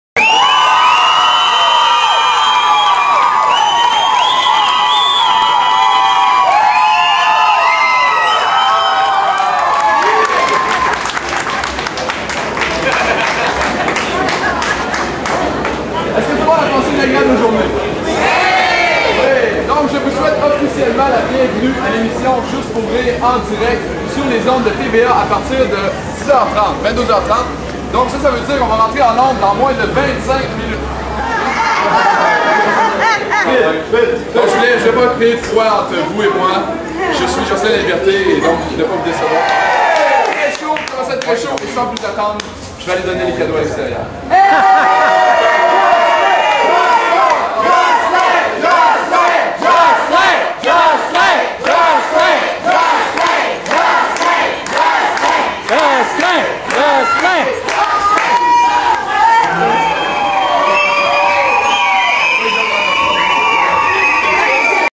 Jocelyn Laliberté en 3-D Anaglyphe
Juste pour Rire En Direct 2004 Jocelyn est Animateur de foule Besoin Lunettes rouge et cyan pour voir en 3-D